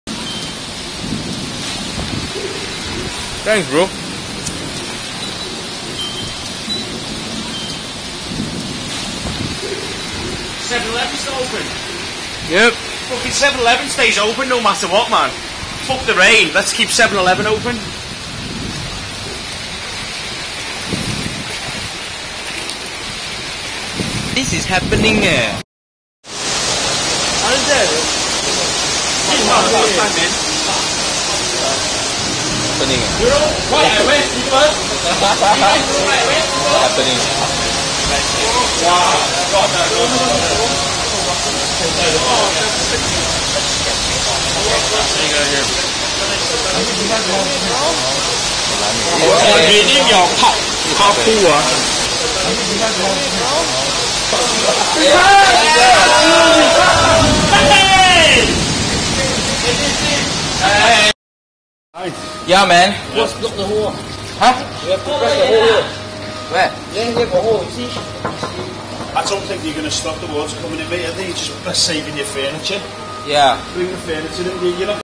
bukit timah road flooded on 5th May 2012, taken at outpost 903
filmed by my brother in law on 5th May 2012